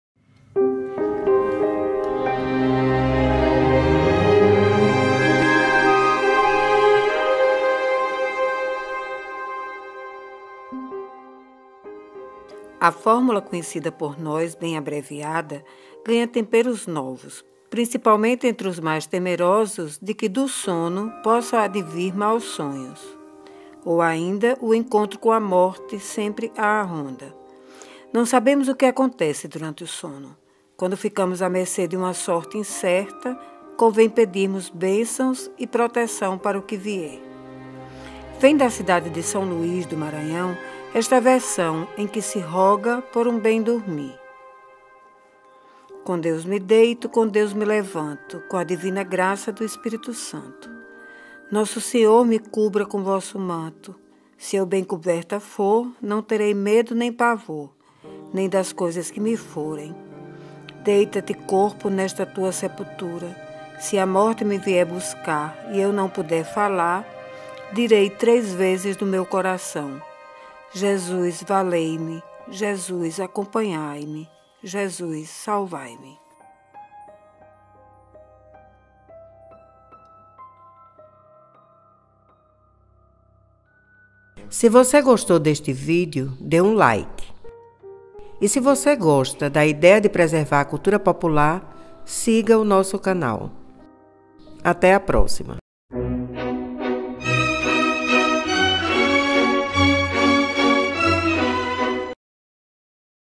Uma oração maranhense para bem dormir
A fórmula conhecida por nós bem abreviada ganha temperos novos principalmente entre os mais temerosos de que do sono possa advir maus sonhos ou, ainda, encontro com a morte, sempre a ronda.
Não sabendo o que acontece durante o sono quando ficamos a mercê de uma sorte incerta, convém pedirmos bênçãos de proteção para o que vier.
Vem da cidade de São Luís, Maranhão, esta versão em que se roga por um bem dormir.
Helenita Monte de Hollanda